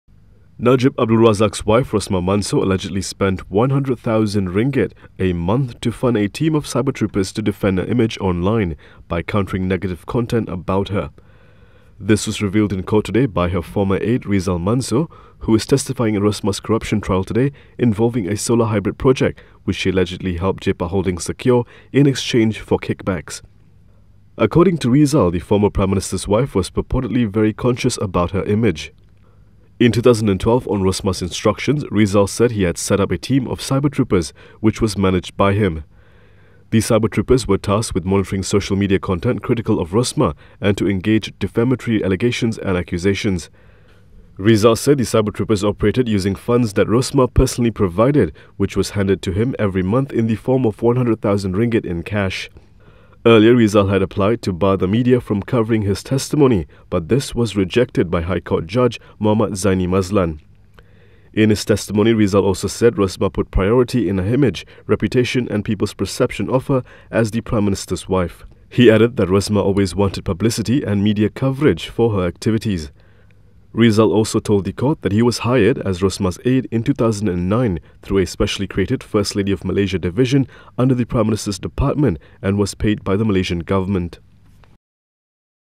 Rosmah allegedly paid cybertroopers RM100k a month to guard her image online
Rosmah Mansor, the wife of former prime minister Najib Abdul Razak, was allegedly very conscious about her image and purportedly forked out RM100,000 a month to fund cybertroopers who were tasked to counter negative content about her on the internet.

This according to her former aide Rizal Mansor who testified in Rosmah's graft trial today involving a solar hybrid project which she allegedly helped Jepak Holdings to secure in exchange for kickbacks.